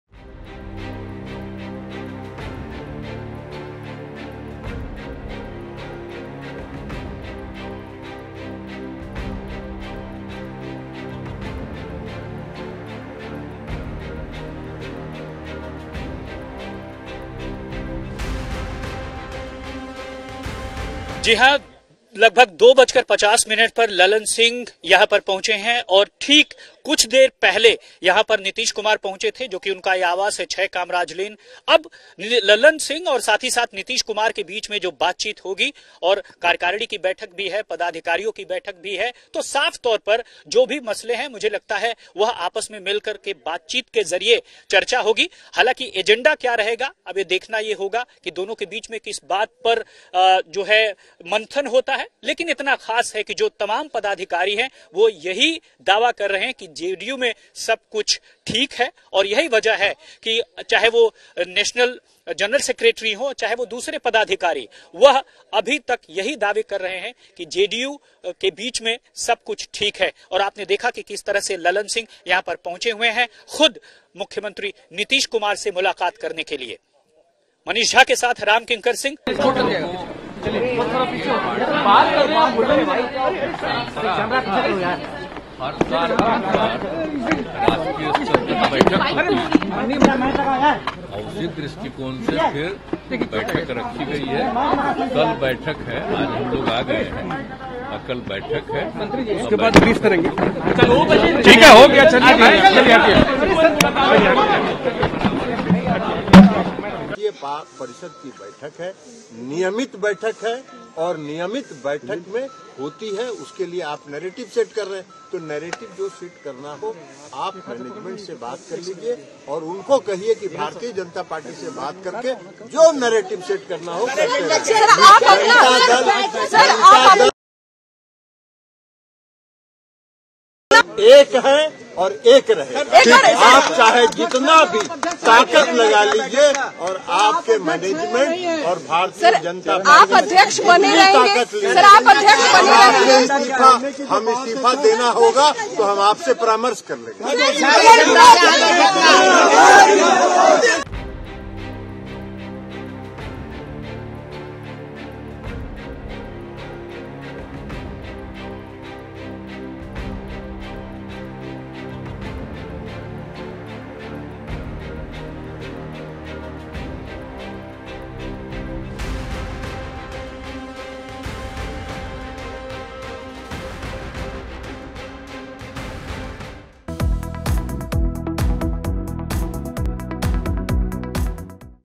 Nitish Kumar और Lalan Singh में अहम बैठक, कभी भी हो सकता है बड़ा फैसला| Bihar Tak
ललन सिंह और नीतीश कुमार की मुलाकात चल रही है. जेडीयू की महत्वपूर्ण बैठक के बीच ये मुलााकात अहम मानी जा रही है.